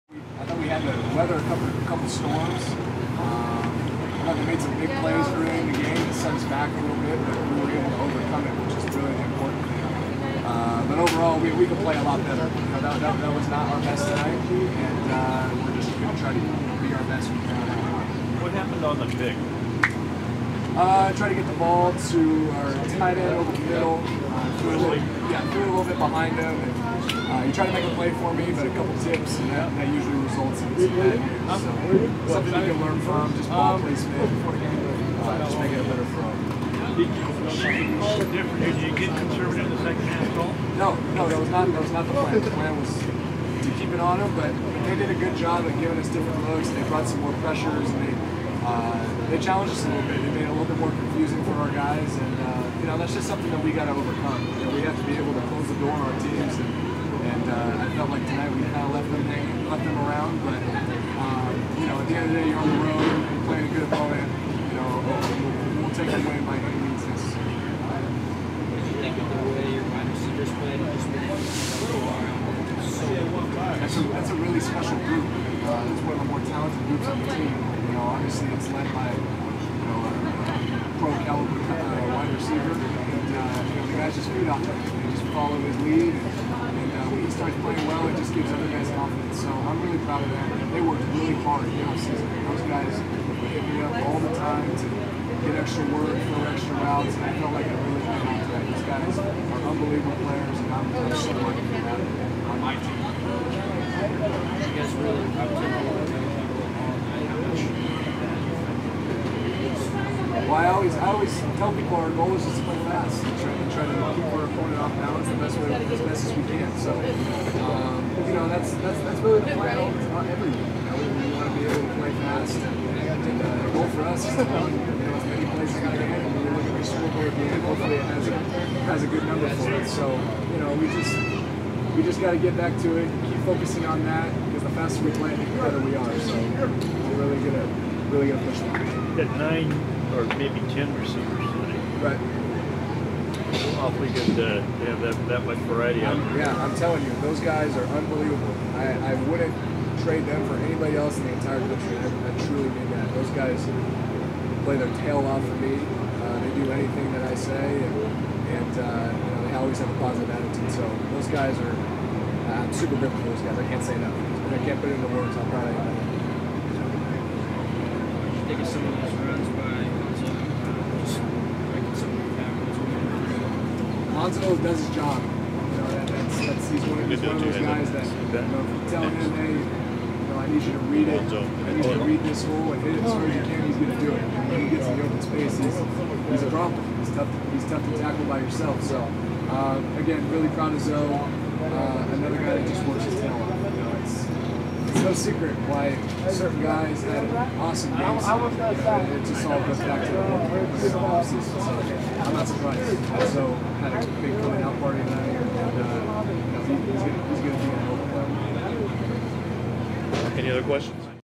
San Jose State University Postgame - Quarterback Jake Maier
Quarterback Jake Maier gives his thoughts on UC Davis Football's victory over San Jose State University on August 30th, 2018.